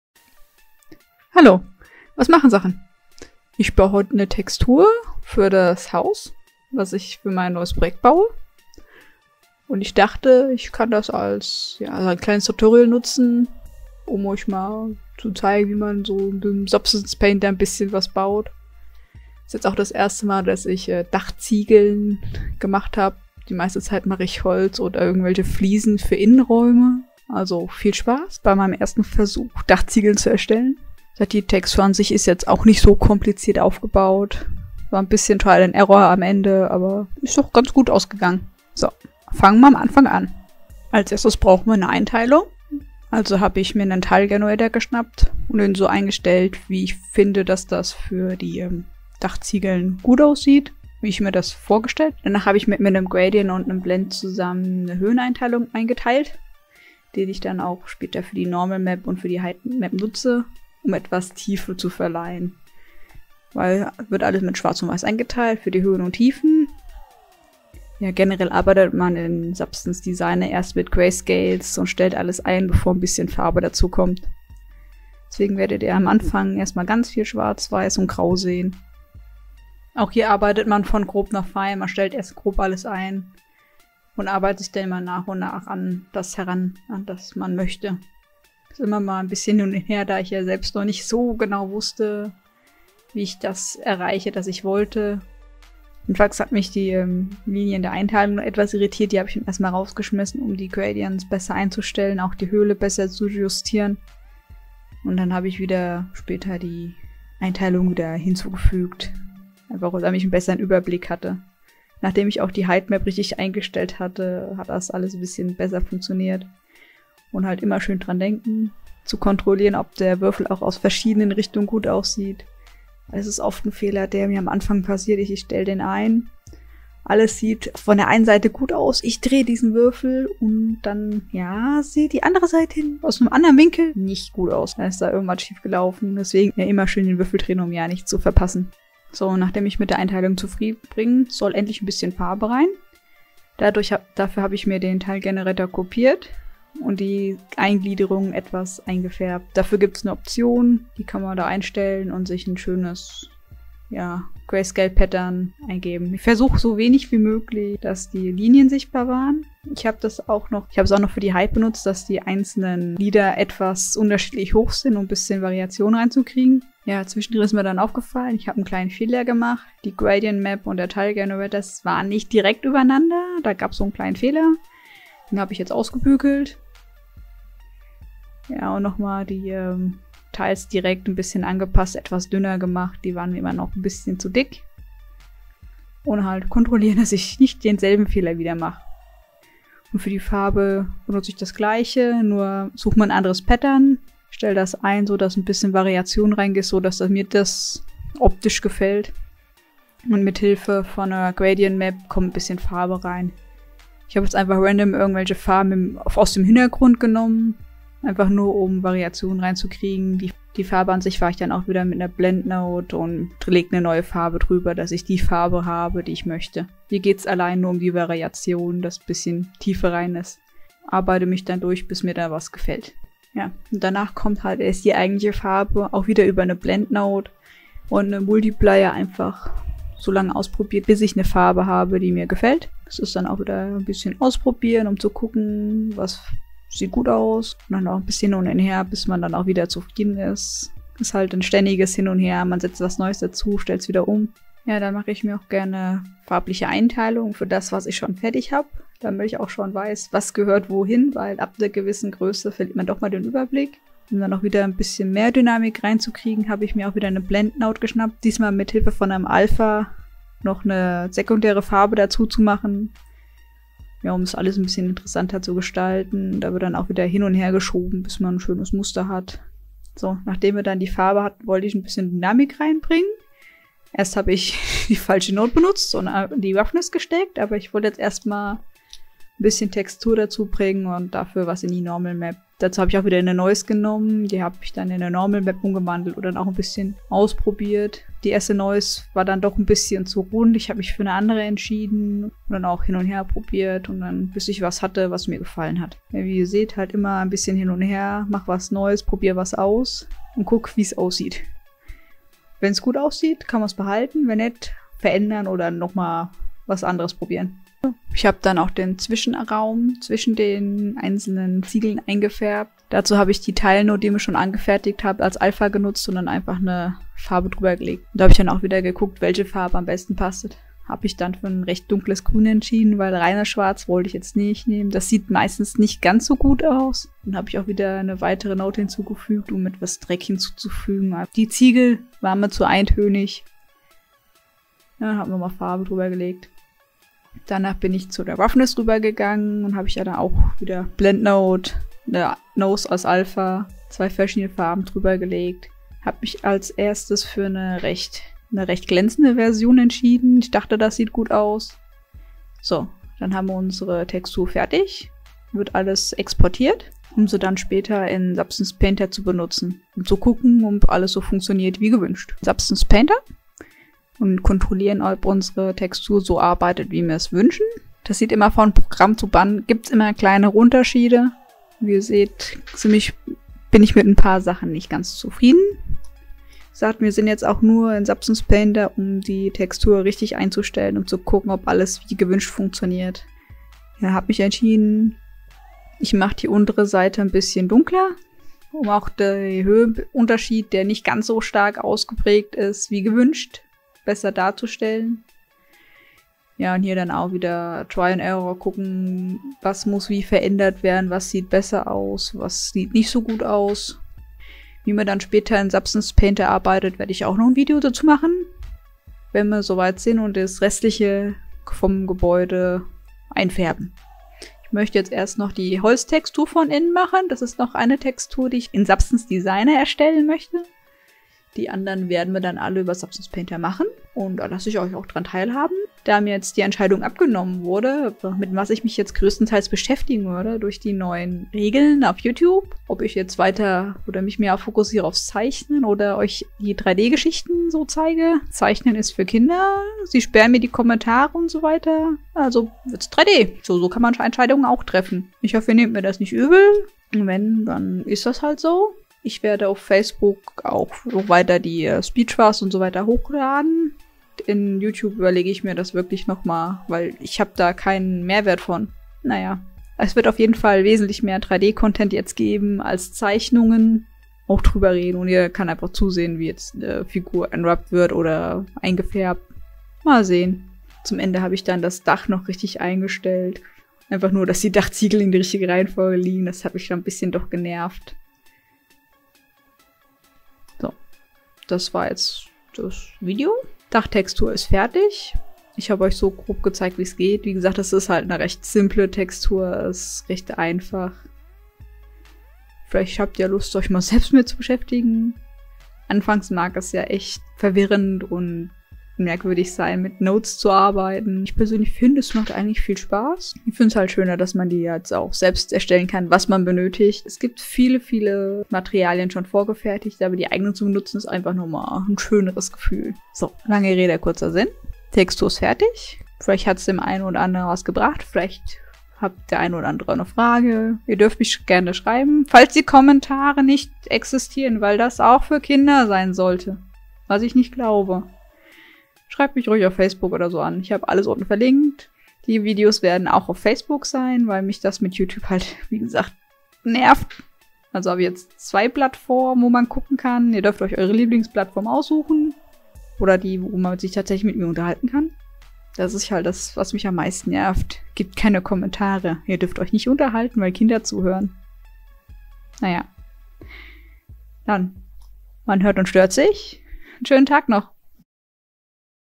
Substance Designer Tutorial (Dach Textur)
Ich erstelle eine Textur für ein Dach und lasse euch daran teilhaben.

Im nachhinein ist mir ein "Fehler" aufgefallen und ich habe ein offset bei dem Titelgenerator hinzugefügt. So sind die Ziegel versetzt angeordnet.